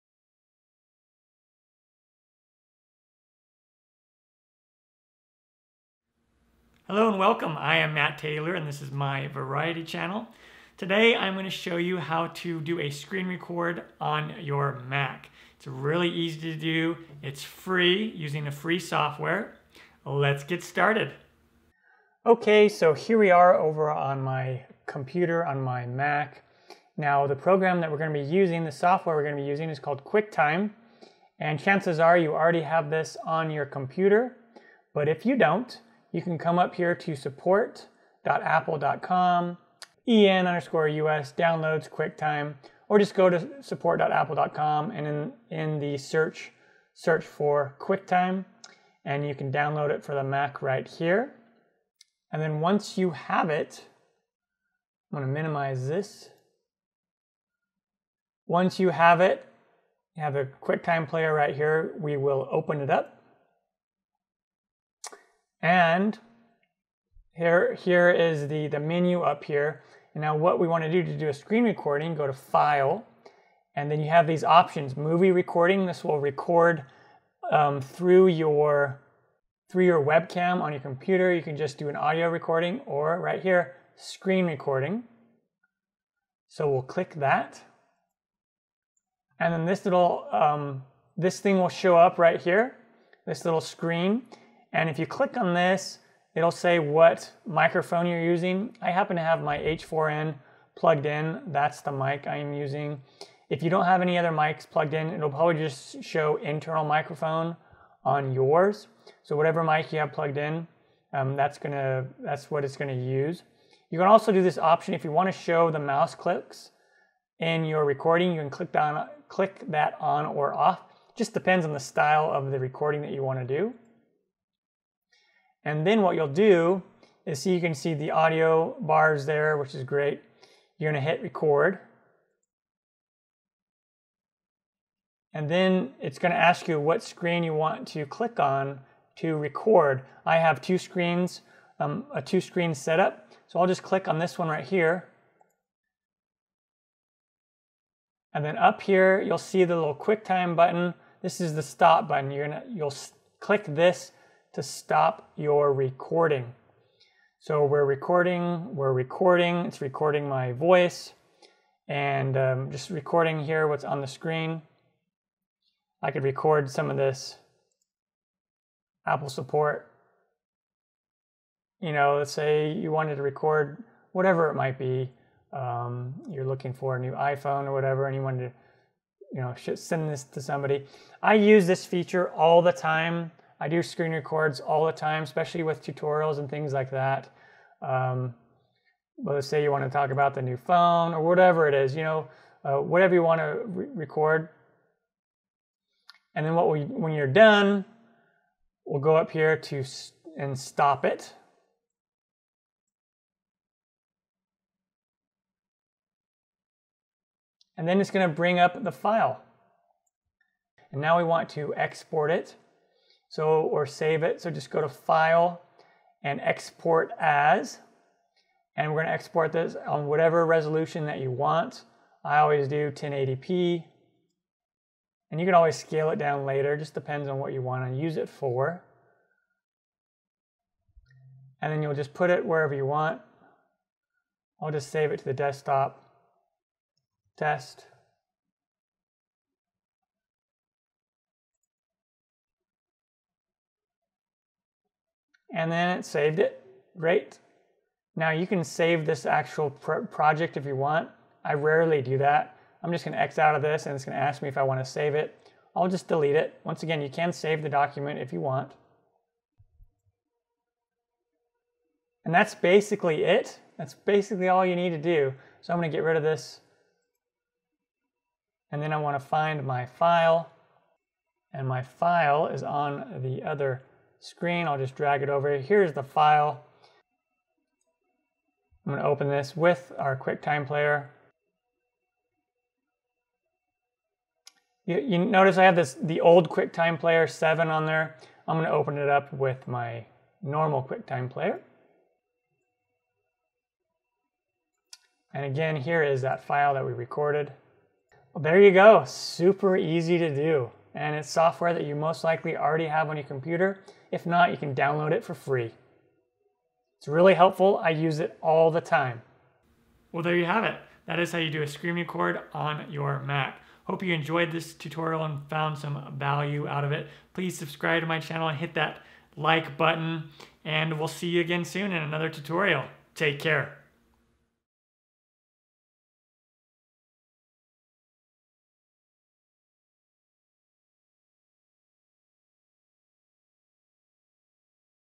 How to Screen Record on MAC for FREE | Easy Screen Capture MAC Tutorial
In this MAC tutorial video I am going to show you how to do a screen record on your MAC for free. Screen recording or screen capture is very easy to do on a MAC using software you most likely already have on your computer called Quicktime. If for some reason you don't have the Quicktime Player you can download it for free. I do screen records or screen captures all the time, and Quicktime makes it super easy. You can also record video and just audio if you like. But in this easy tutorial I am only talking about the screen recording function. Let's get started!

If you have any questions or comments leave them down below and I will get back to you as soon as I can. :)

Video Production Gear I use:
OBS Studio